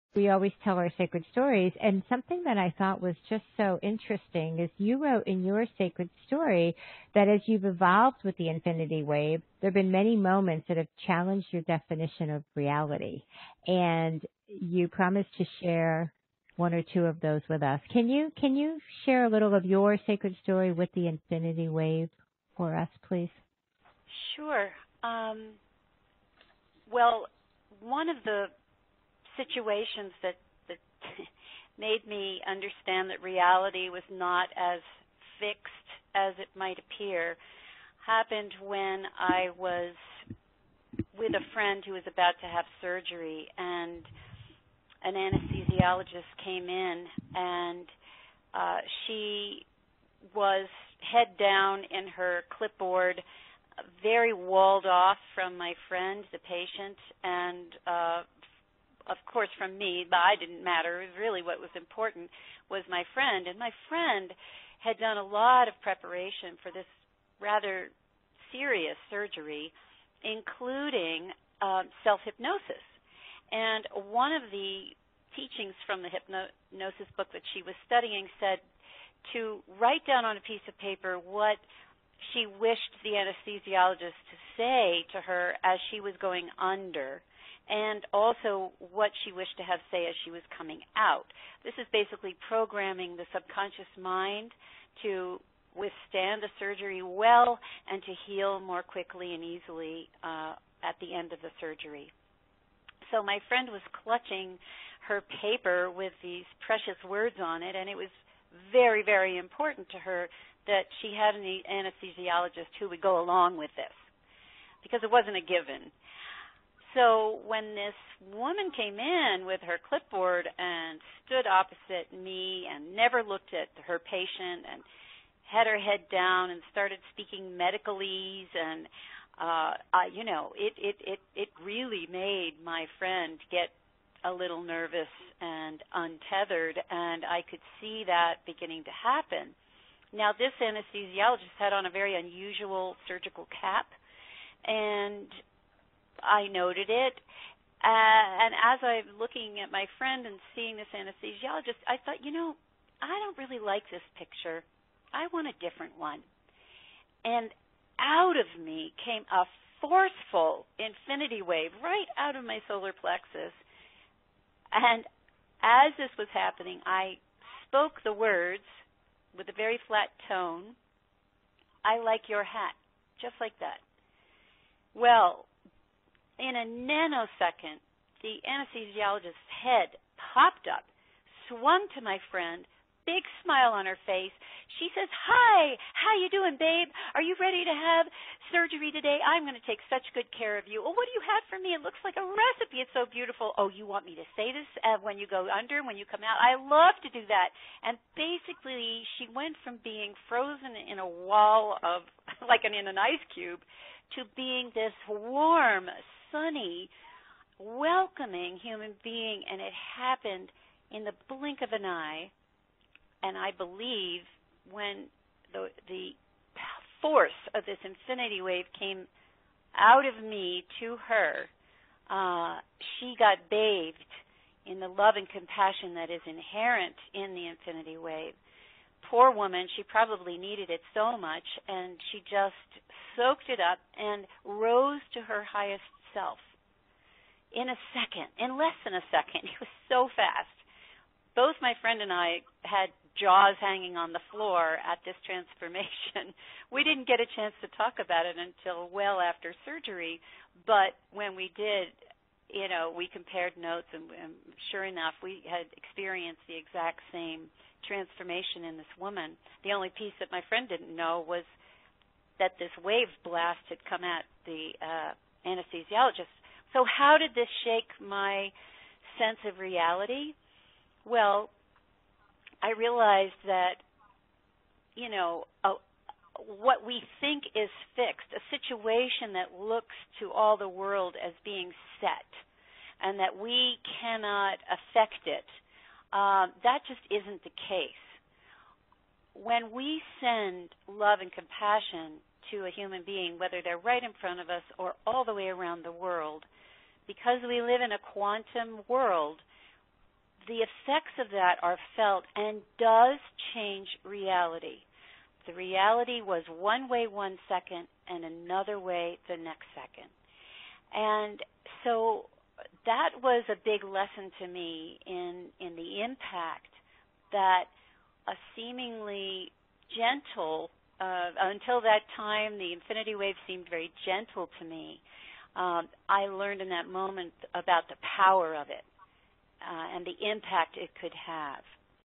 Sacred Stories Hope Fitzgerald
What we think is fixed is not always the case! Listen to Hope Fitzgerald's Sacred Story on using the 10th Dimensional Gift of the Infinity Wave to shift the reality of her situation.  Listen to our full Sacred Stories interview with Hope on what the Infinity Wave is and the power and beauty of using the Infinity Wave yourself and check out Hope's powerful offer of 19+ expanded practices you can do with the Infinity Wave to assist YOU in expanding your spiritual consciousness!